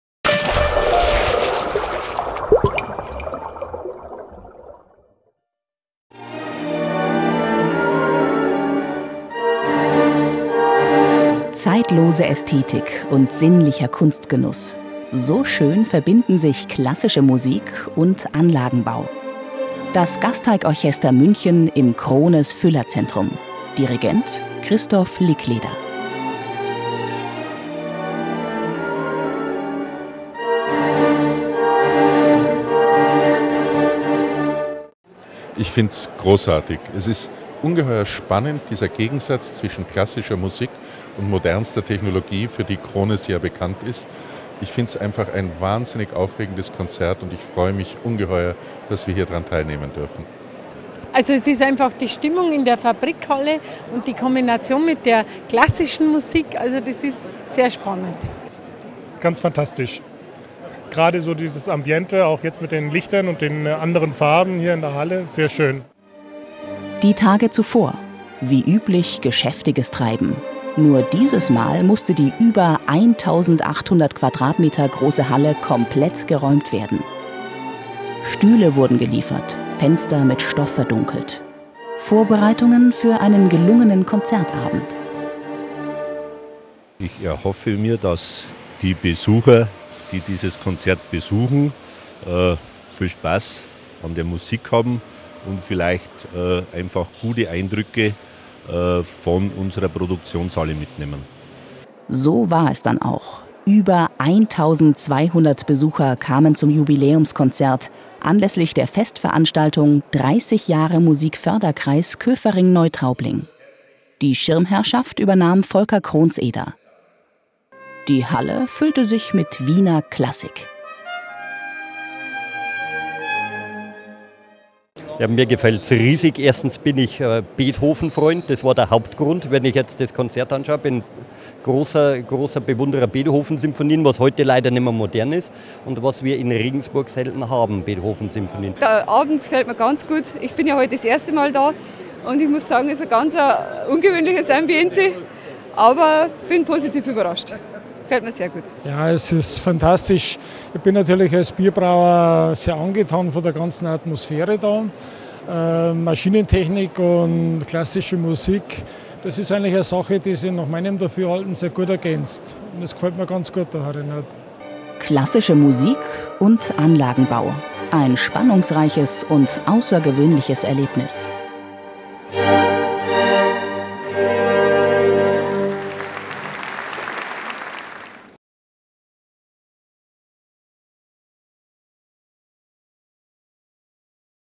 Krones: Wiener Klassik trifft Technik
Das Gasteig-Orchester München im Krones Füllerzentrum. Über 1200 Besucher kamen zum Jubiläumskonzert anlässlich der Festveranstaltung "30 Jahre Musikförderkreis Köfering-Neutraubling". Die Schirmherrschaft übernahm Volker Kronseder. Die Halle war erfüllt von Mozart, Haydn und Beethoven. Klassische Musik und Anlagenbau - ein außergewöhnliches Erlebnis.